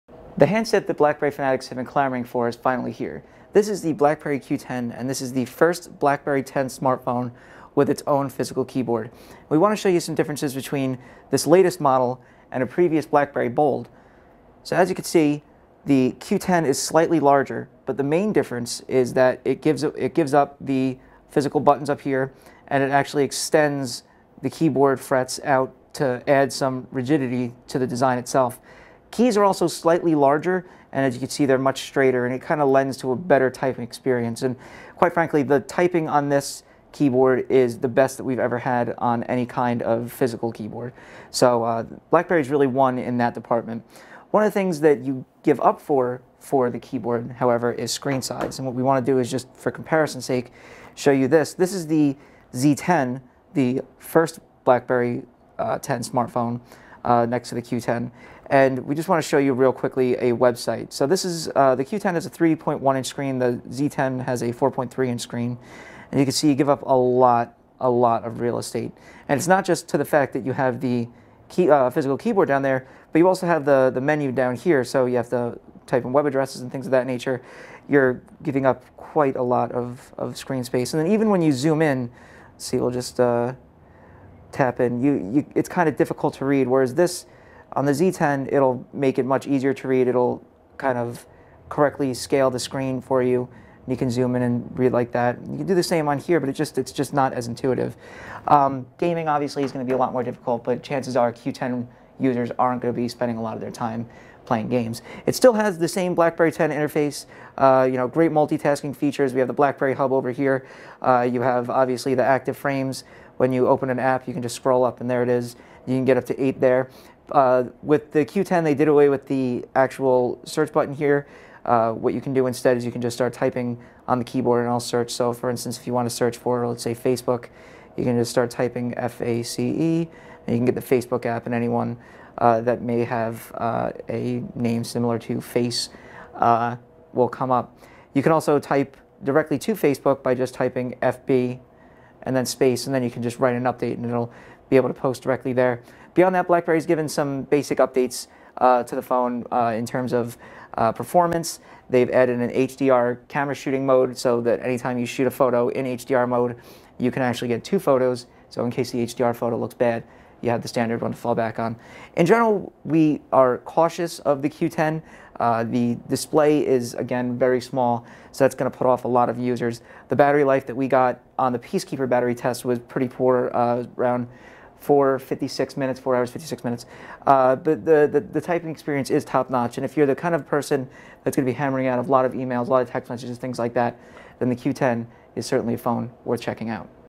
Blackberry Q10 Hands On
We go with the blackberry Q10, the first Blackberry 10 device with a qwerty keyboard.